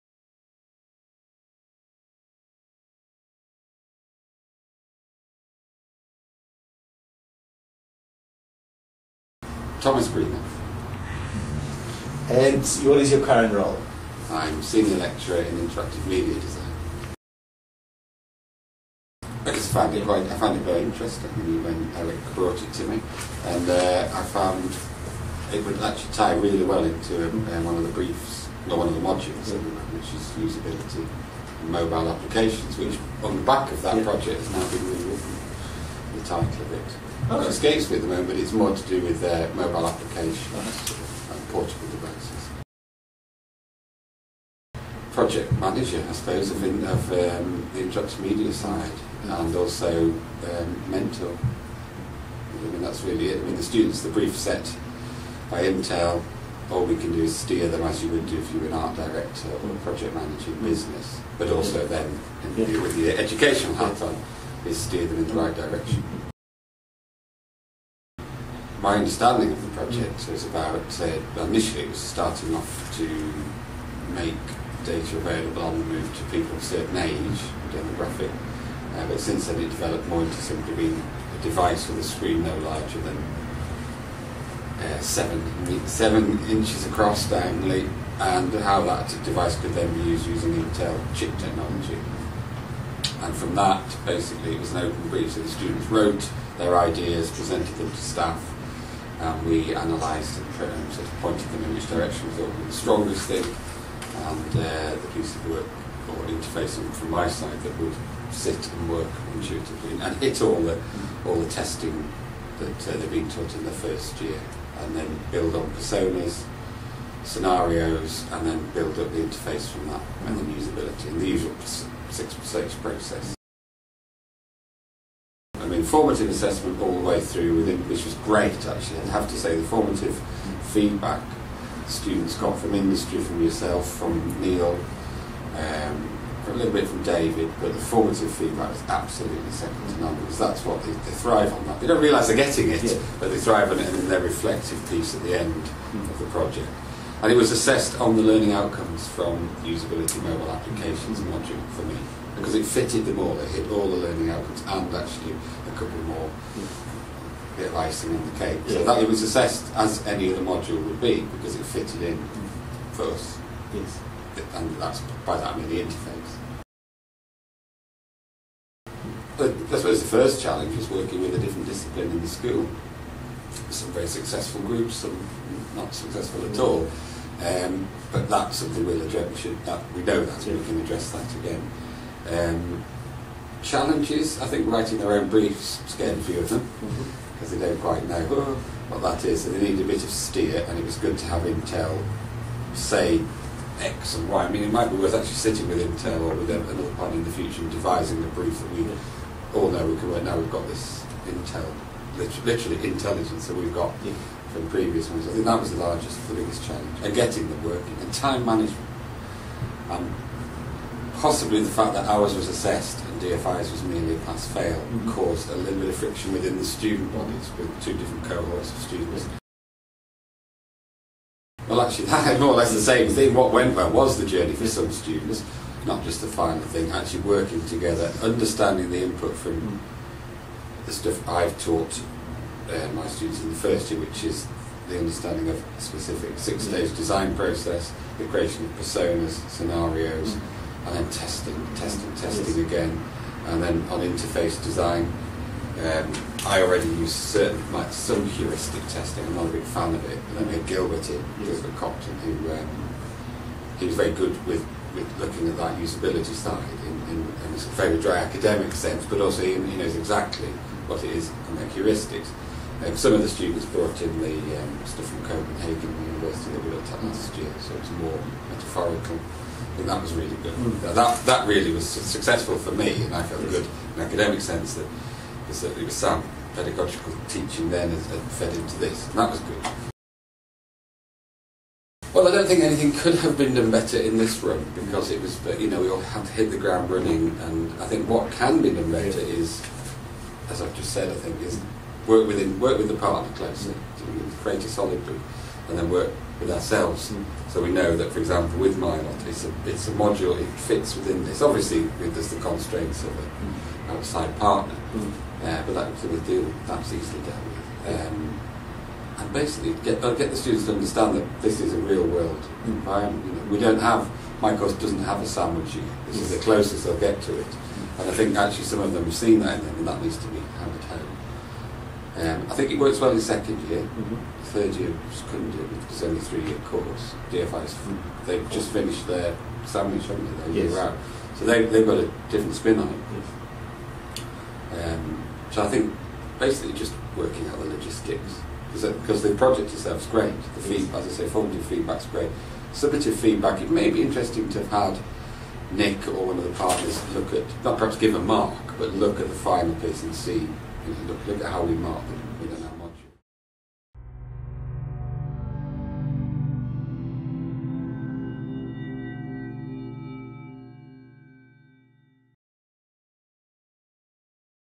The Global Studio 2009: On the Move 2 - Thomas Greenough reflections
Thomas Greenough, Collaborative Partnerships Manager and Senior Lecturer, Interactive Multimedia Design at the School of Design at Northumbria University, reflects on the Global Studio's projects: 'On the Move 2: Computing Now' a collaborative project supported by Intel.

... 'formative assessment all the way through, which is great actually, and I have to say, the formative feedback students got from industry, from [other academics], but the formative feedback was absolutely second to none because that's what, they thrive on that and they don't realise they're getting it but they thrive on it in their reflective piece at the end of the project.'